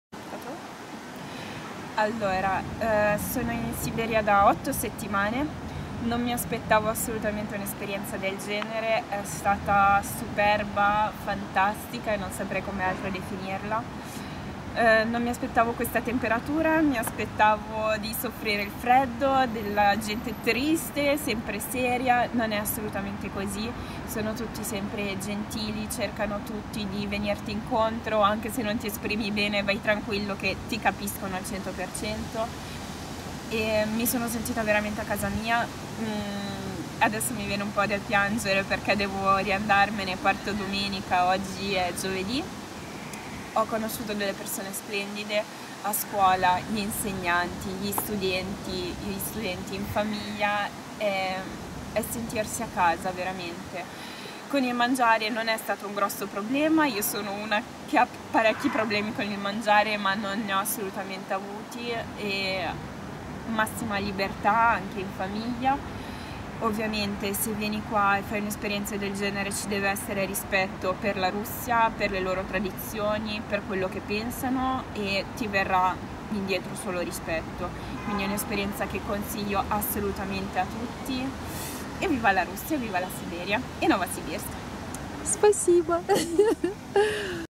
Review (in Italian) of Exlinguo Novosibirsk Russian courses by Manuela, Italy | July 2015
Testimonial of Manuela, Italy, talking about her experience at Exlinguo Novosibirsk.
Manuela came study with us from June 1st until July 24th 2015. Starting from level A2+, she reached level B2 within 8 weeks with our course.
Спасибо, Мануэла, за ваш отзыв! До скорой встречи в России!